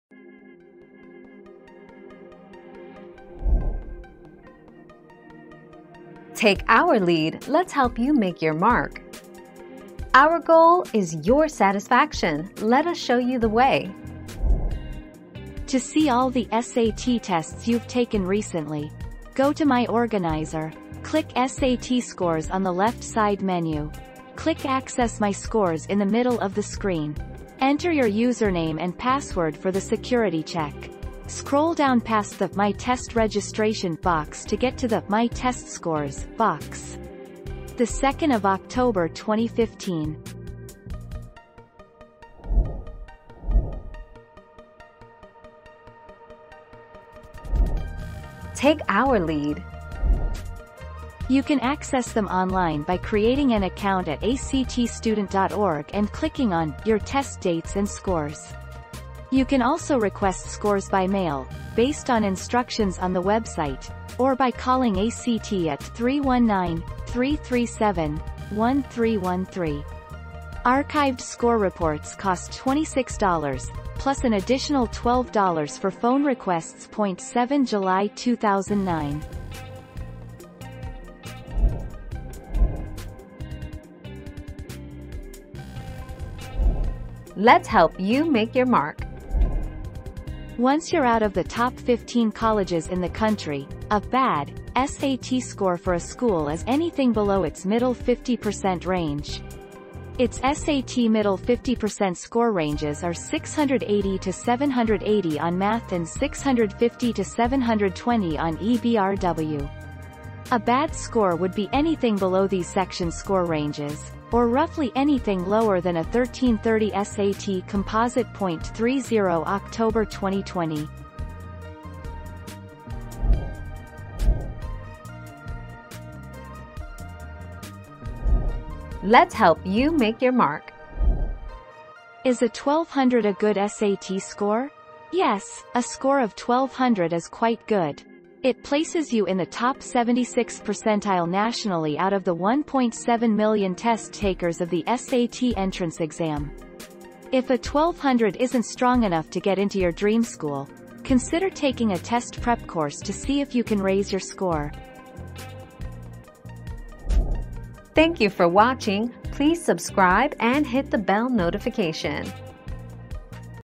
How do I find my SAT scores online?
00:00 - How do I find my SAT scores online?
00:44 - How can I find my old SAT scores online?
01:23 - What is a bad SAT score?
02:04 - Is 1200 a good SAT score?

Laura S. Harris (2021, March 20.) How do I find my SAT scores online?
    AskAbout.video/articles/How-do-I-find-my-SAT-scores-online-231346